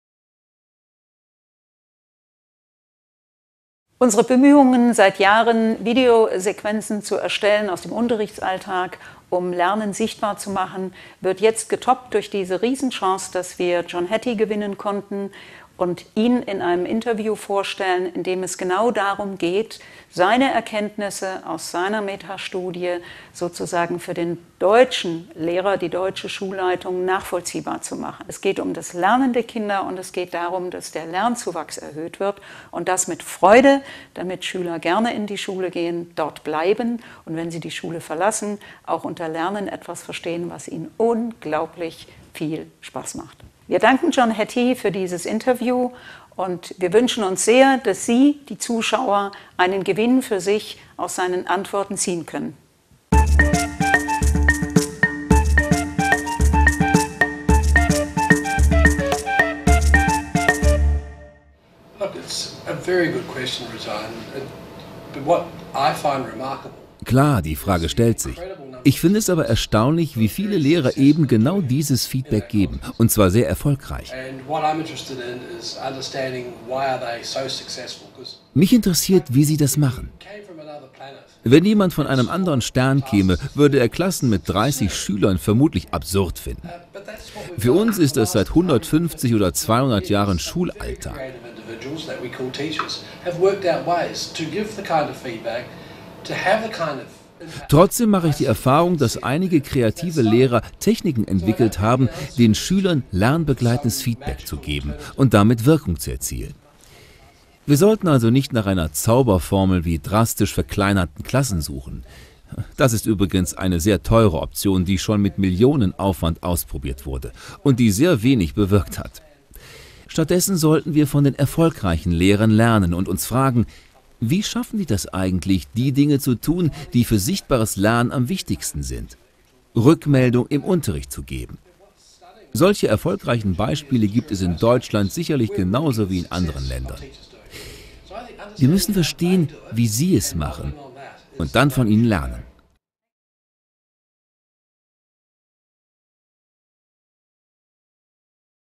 Frage 1
Regine Berger im Interview mit John Hattie: 8 Fragen an John Hattie